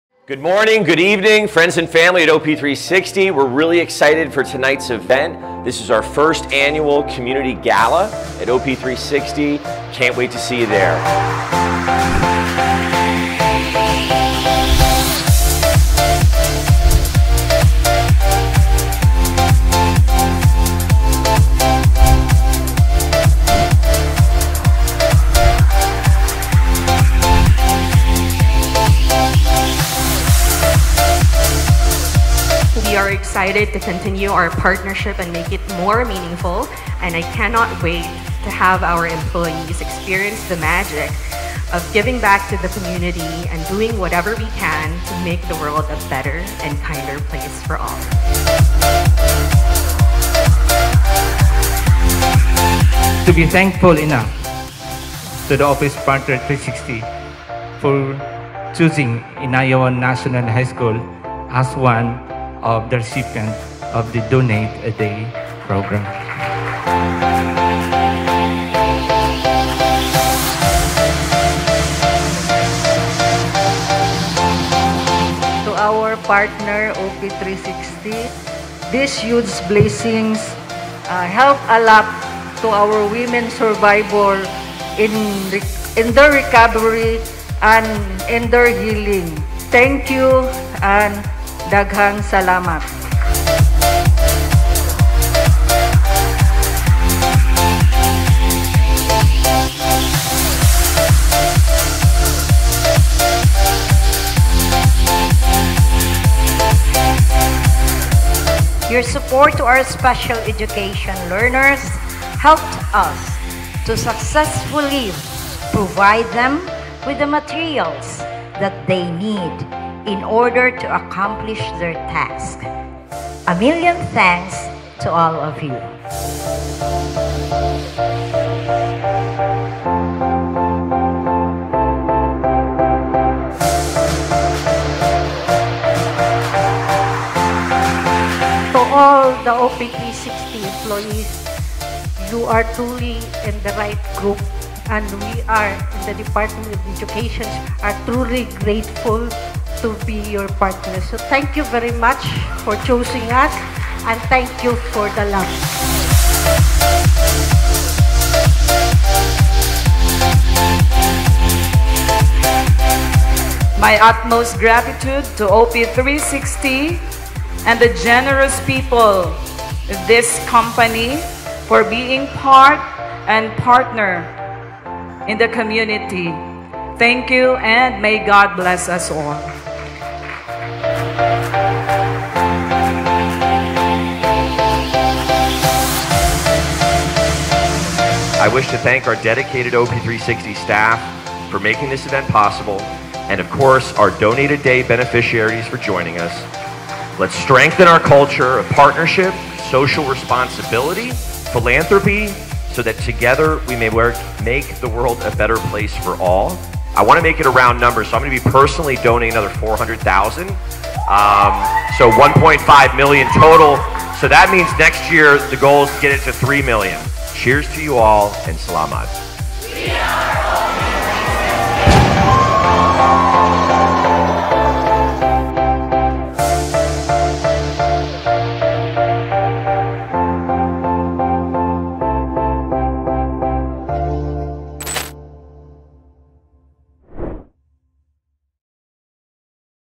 OP360 Community Gala - A Night with our DonateADay Partners
A night of community building, social impact, and fun with our DonateADay community partners as we launched the OP360 2022 Philanthropy Leave and awarded over Php1.5M in financial grants to support the priority projects of our 5 amazing beneficiaries

1. Don Vicente Rama Special Education Center
2. Inayawan National High School
3. Hipodromo Elementary School
4. Regional Haven for Women & Children
5. Estaca Integrated School